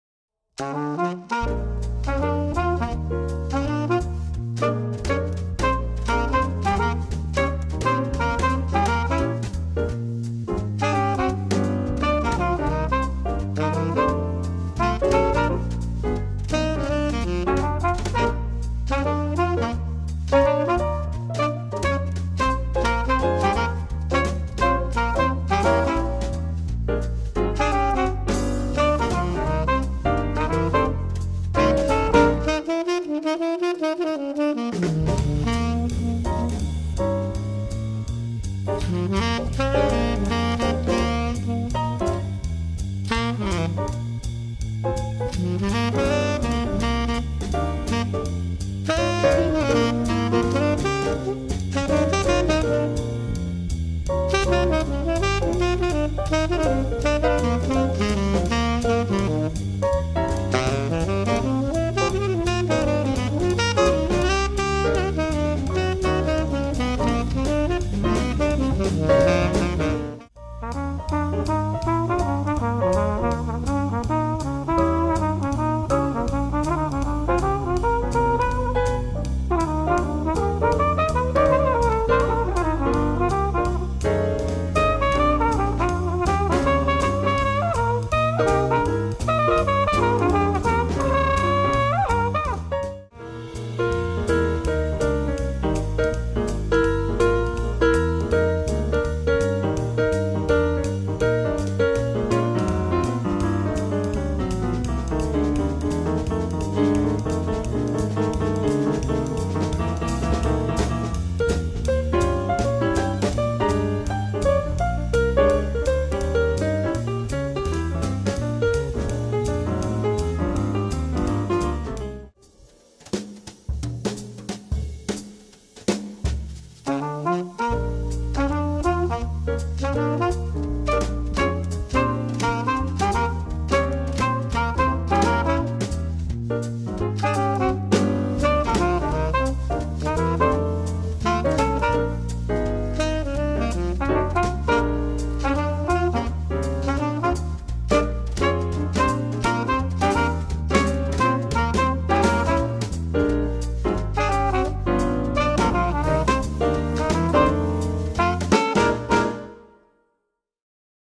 Jazz 4 All, "Promo Clip"
Jazz 4 All Videoclip promocional con short bios e información básica del cuarteto.